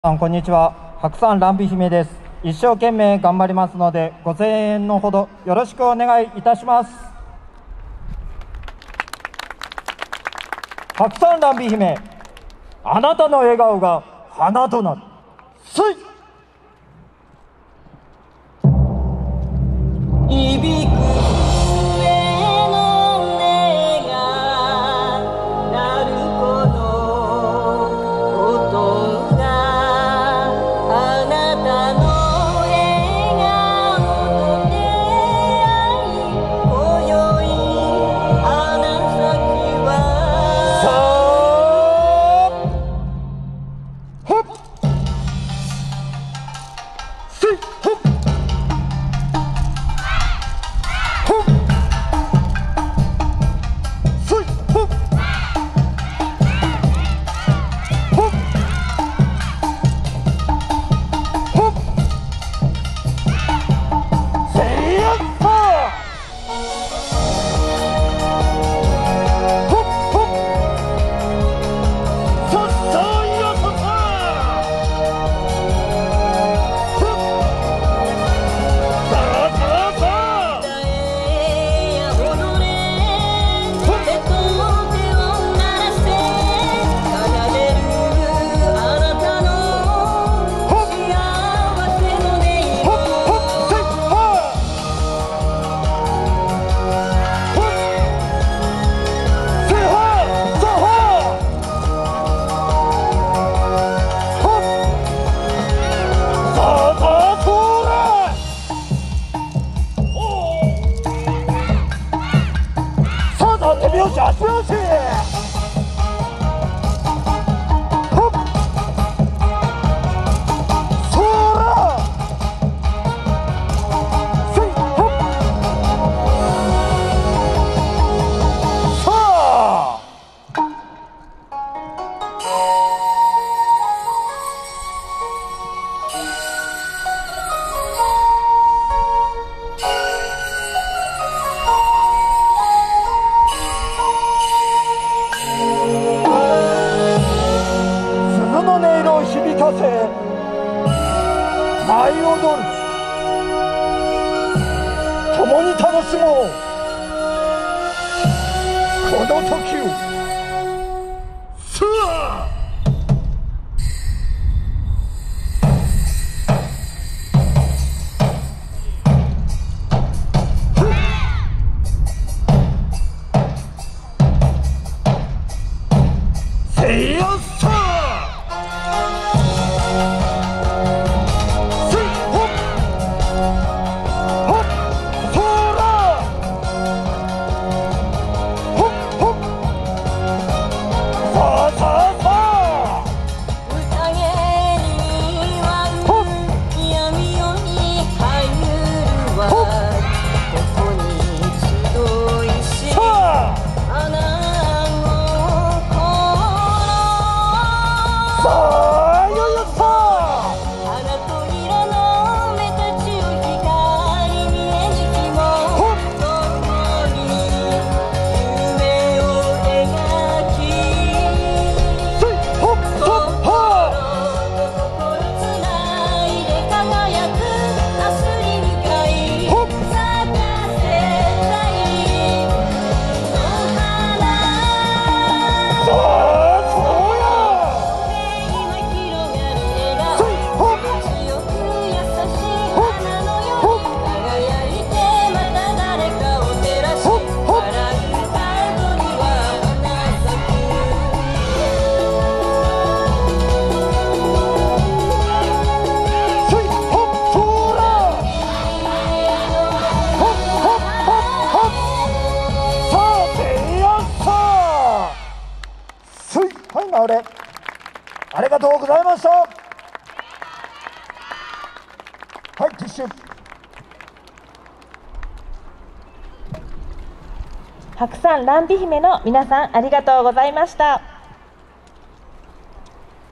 [4K] 白山蘭美姫 1回目 第22回YOSAKOIソーラン日本海本祭 2021
第22回YOSAKOIソーラン日本海本祭
2021年10月24日(日)
宝達志水運動公園